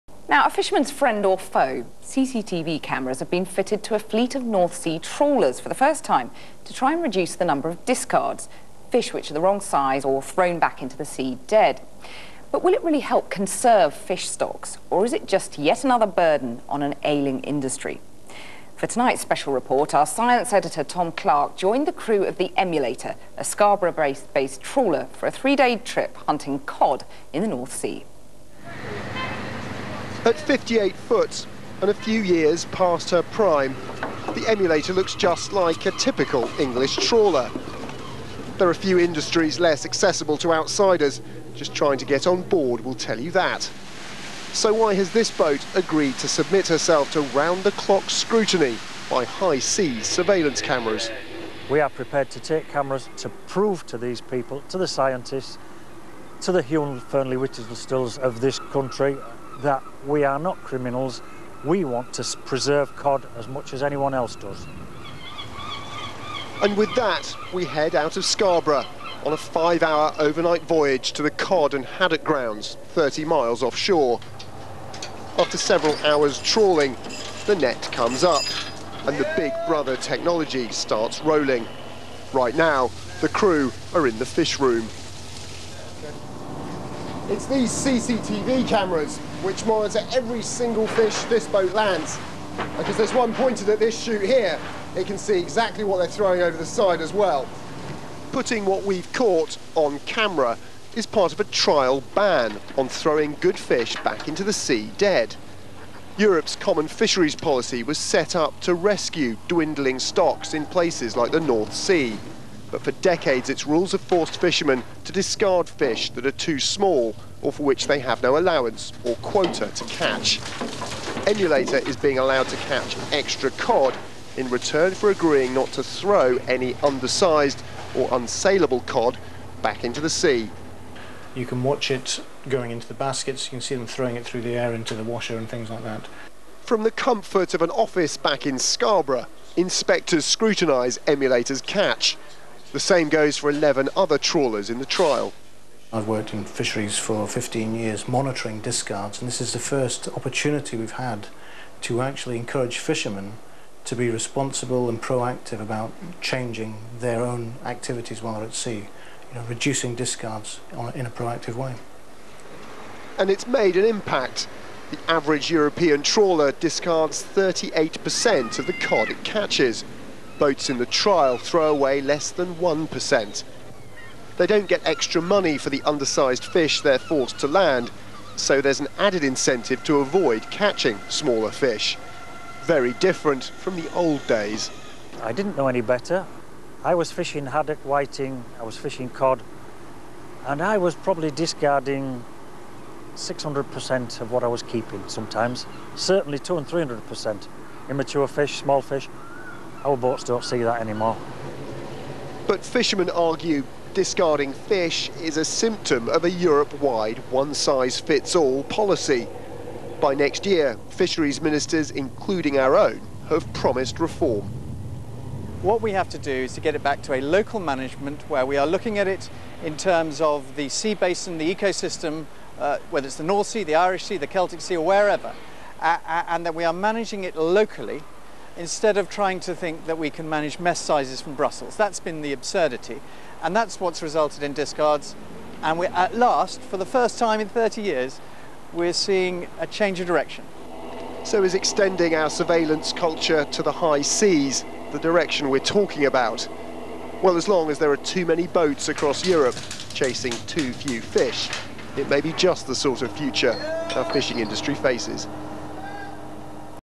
North Sea Fishing CCTV trial reduces discards to 0.2%
A new CCTV system for trawlers aims to keep any eye on fishermen in a bid to stop "discards" - throwing back fish when a catch exceeds the quota. Channel 4's Tom Clarke reports.

At 58 foot and a few years past her prime, Emulator looks just like many other English fishing trawlers.

Only she has been fitted with experimental CCTV technology in the hope of ending the perverse practice of "discards" - of throwing good fish back over the side dead.

Not every trawler owner would consent to having Big Brother-style cameras fitted to his boat. But Fred Normandale, who part-owns Scarborough-based Emulator, sees it as an opportunity to show what really goes on at sea.

"We're prepared to take cameras to prove to the scientists and the Hugh Fearnley-Whittingstalls of this country that we are not criminals and we want to preserve cod as much as anyone else does."

The dig is aimed at the celebrity chef whose TV campaign helped force European fisheries ministers agree to a ban on discards. Earlier this month, they agreed to ban discards of all fish under the common fisheries policy by 2018.

90 per cent discards

A basic flaw the CFP - set up in the 1980s to protect fish stocks - sees much of the fish European boats catch go straight back over the side.

The average European fishing trawler discards 38 per cent of its catch - for some species of fish 90 per cent are thrown away. But boats like the Emulator, involved in the current trial, have reduced discards to just 0.2 per cent.

The problem of discards results from the way fishing rights are shared out. Each vessel is awarded a strict quota in tonnes for the fish species it wants to catch. If it catches too much of that species, or members of another species, it faces a fine for landing them. Fishermen get round the problem by dumping the fish they can't land back into the sea.

The trial is for boats fishing cod in the North Sea and sole in the English channel.

It requires fishermen to land all the fish they catch. If they have caught undersized fish, they are unable to sell them, and if they meet their quota allocation, they have to stop fishing. In return for those conditions, fishermen are given more fishing quota than equivalent boats.

Black box monitoring

Compliance is tested by four CCTV cameras fitted to the boats. One monitors the catch coming over the side, others watch which fish are kept and which are discarded.

The video, along with GPS data on exactly where the boat is fishing, is then stored on a black box which is sent to the Marine Management Organisation's monitoring centre back in Scarborough.

"It's not so much an intrusion as an incentive," said Grant Course, the trial manager with the MMO. "This encourages fishermen to be responsible for his own resource."

According to the MMO boats in the trial take more effort to avoid catching undersize fish which have no value at market, but under the conditions of the trial they are forced to land.

Reform priority

Banning discards is high on the list of reforms being negotiated today by the EU.

UK Fisheries Minister Richard Benyon said the British government, with the support of Denmark which is chairing the fisheries council, wants to see a ban in place by 2014. "This is a once in a decade opportunity to get this right," said Benyon.

But even if the minister can make discards ban work in British waters, it will not work unless it is adopted Europe-wide. The proposal will face tough opposition from France and some Mediterranean states who benefit most from the CFP in its current form.

"What we have to do is get it back to local management instead of thinking that we can manage mesh-sizes from Brussels. That's been the absurdity and at last we're seeing a change in direction.